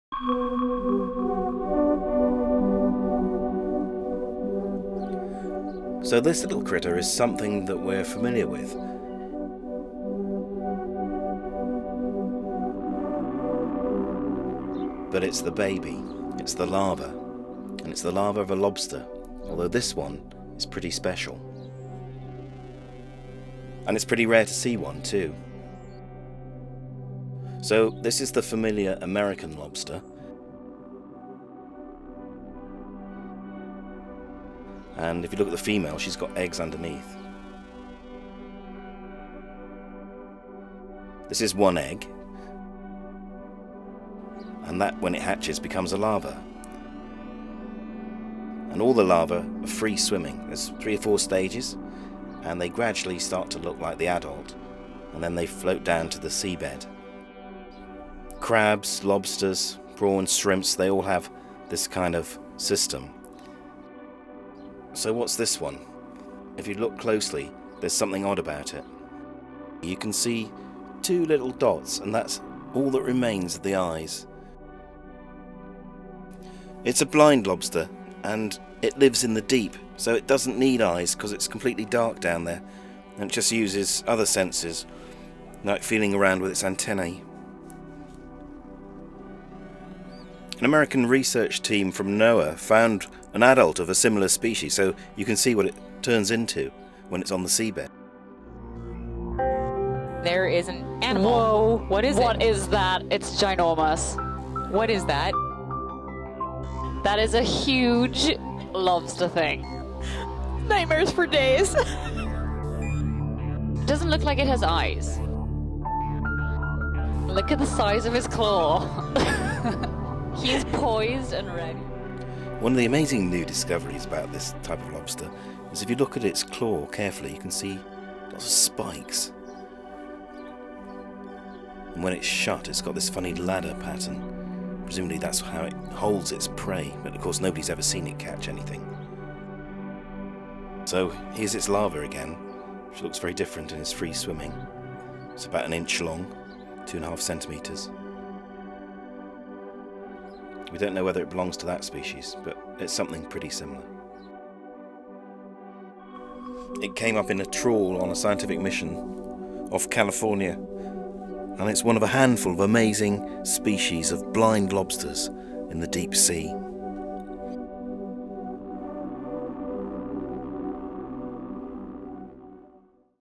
Strange claw of weird blind lobsters
Blind lobster larvae, Stereomastis sp. live in burrows on the deep seabed and are difficult to find. Only recently was the link made between larvae and adult and its astonishing pronged claws. This young blind lobster was found off California at about 600m.

American Lobster video source, Wikimedia Commons citation:

Description: English: American lobster (Homarus americanus) in the Atlantic. In this video, sections of the original material have been removed, the audio has been removed and slow-motion replays have been inserted.
Author Justin Pater

Drawings: Herrick, Francis Hobart (1911) Natural History of the American Lobster, Bulletin of the Bureau of Fisheries, vol.29, 1909, Washington - public domain images.